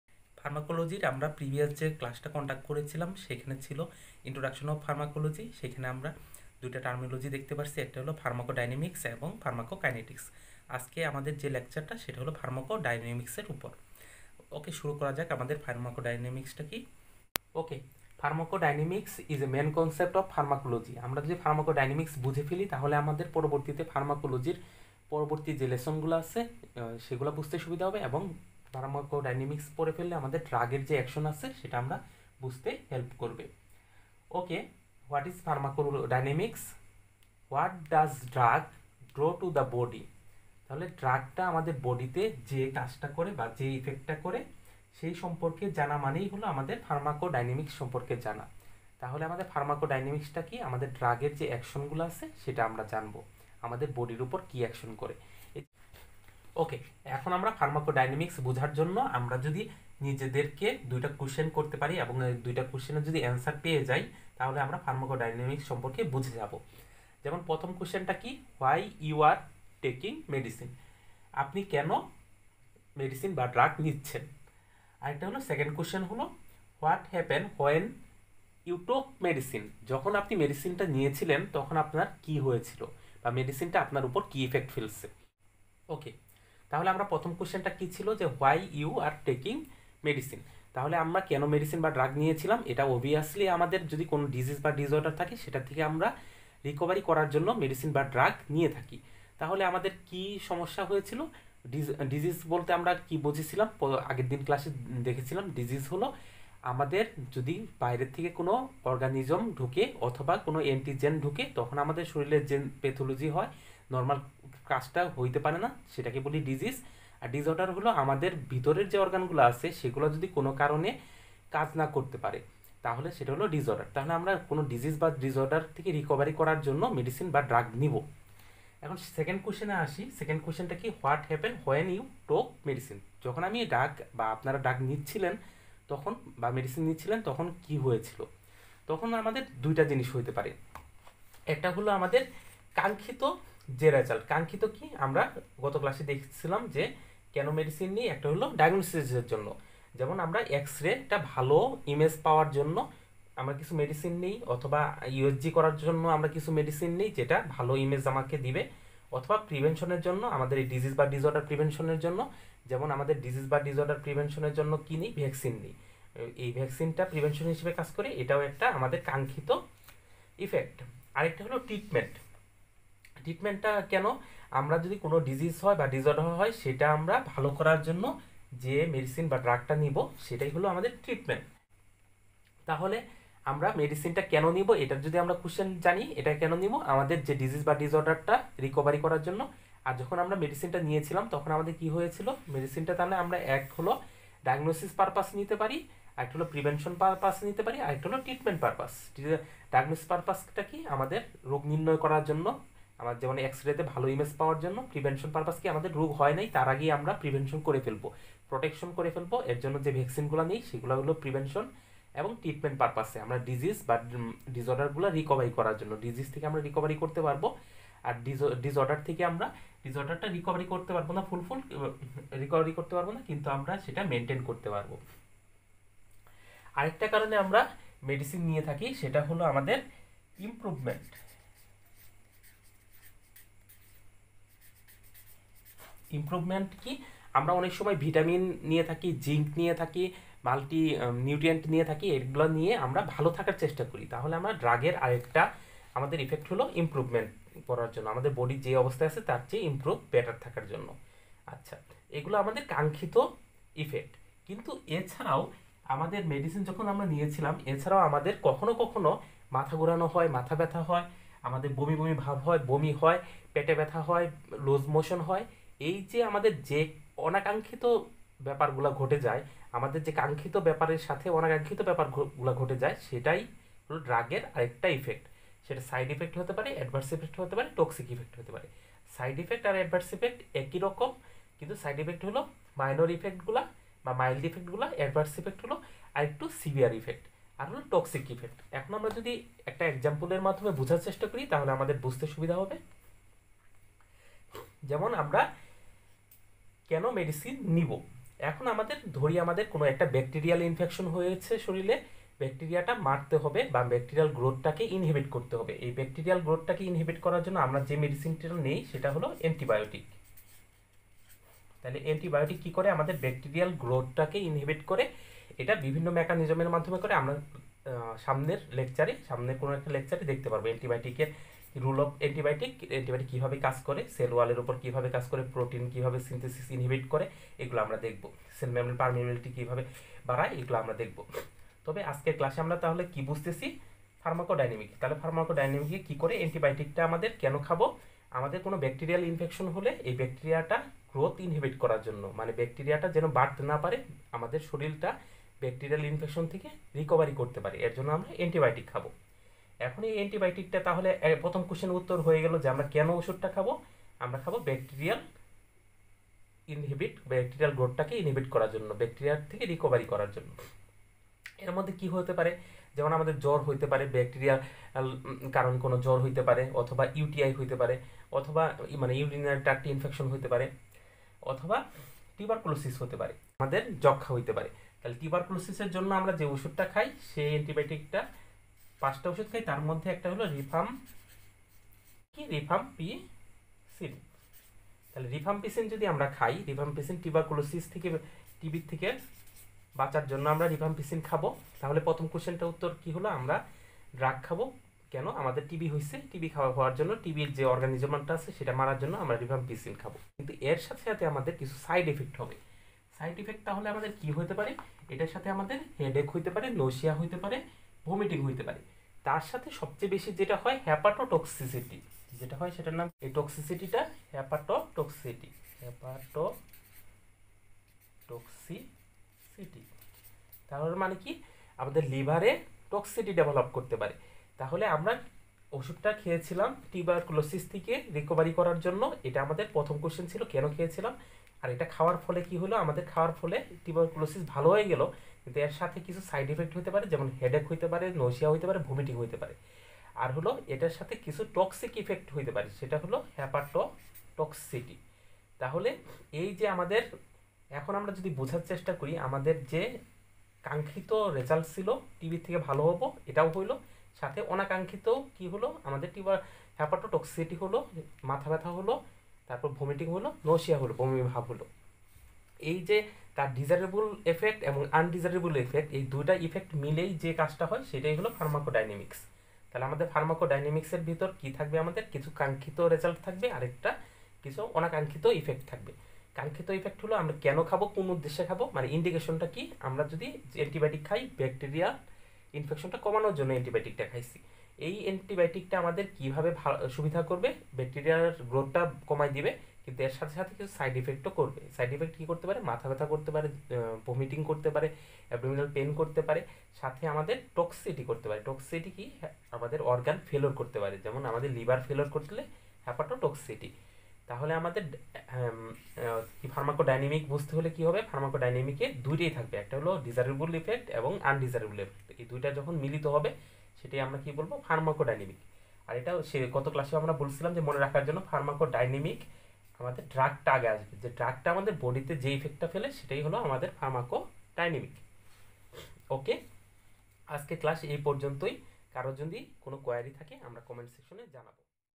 Pharmacology Lecture 0 2: - PHARMACODYNAMICS (MADE EASY) in bangla
Pharmacodynamics is the study of the biochemical and physiological effects of drugs on the body, often described as “what the drug does to the body.”